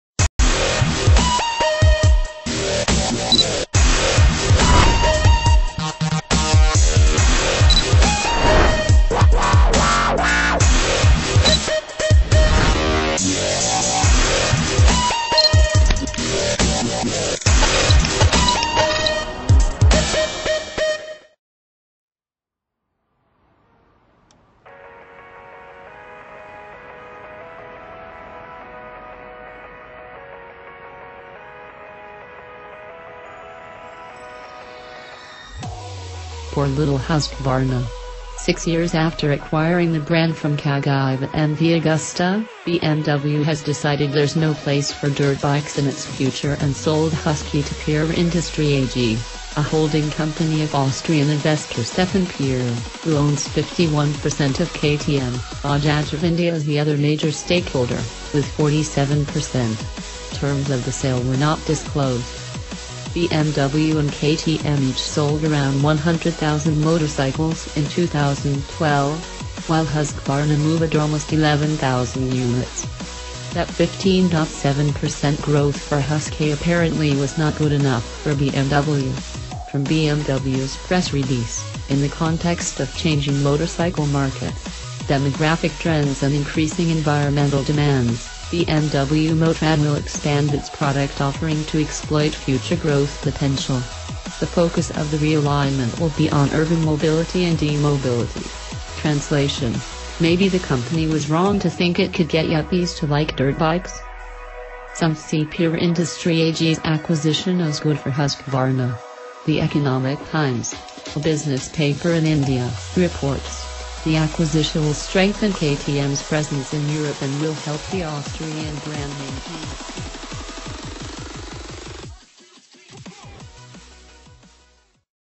HOT NEWS !!! BMW Backed Husqvarna to Launch Five New Road Bikes Performance Review
Poor little Husqvarna. Six years after acquiring the brand from Cagiva MV Agusta, BMW has decided there’s no place for dirtbikes in its future and sold Husky to Pierer Industrie AG, a holding company of Austrian investor Stefan Pierer, who owns 51 percent of KTM (Bajaj of India is the other major stakeholder, with 47 percent). Terms of the sale were not disclosed.